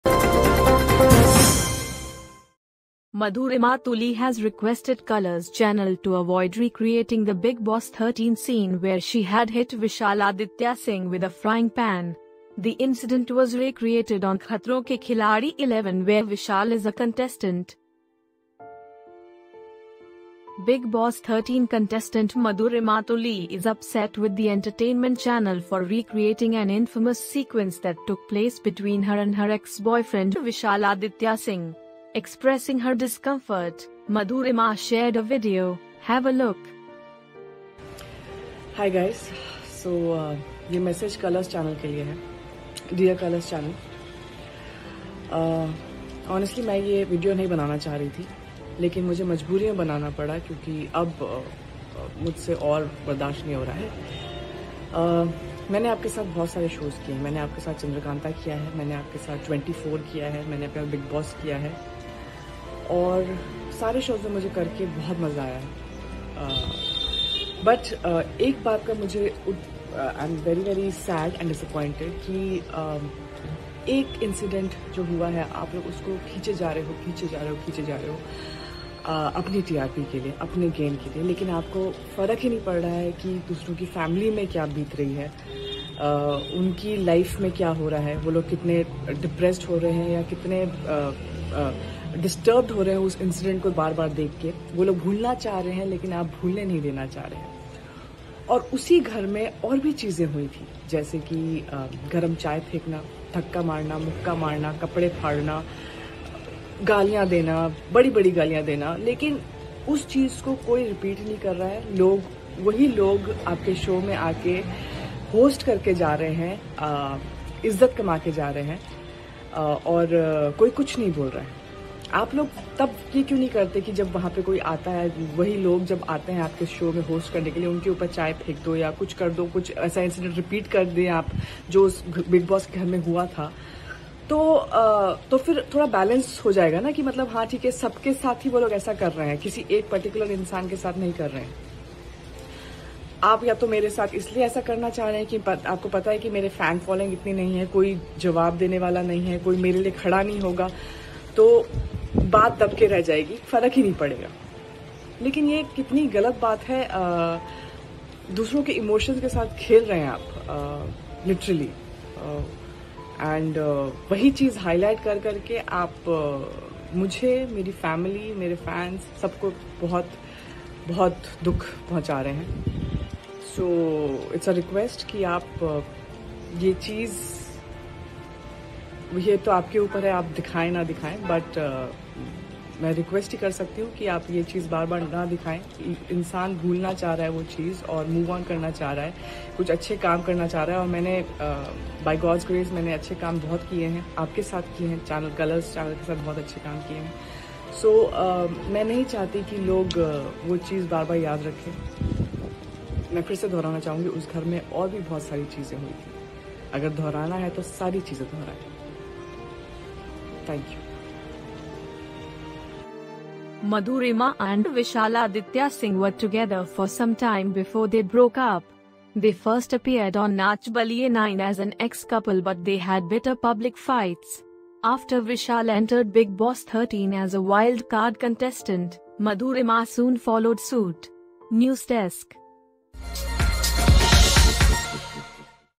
Watch, Madhurima Tuli urges Colors not to recreate frying pan incident with ex Vishal
Madhurima Tuli has "requested" Colors channel to avoid recreating the Bigg Boss 13 scene where she had hit Vishal Aditya Singh with a frying pan. The incident was re-created on Khatron Ke Khiladi 11 where Vishal is a contestant.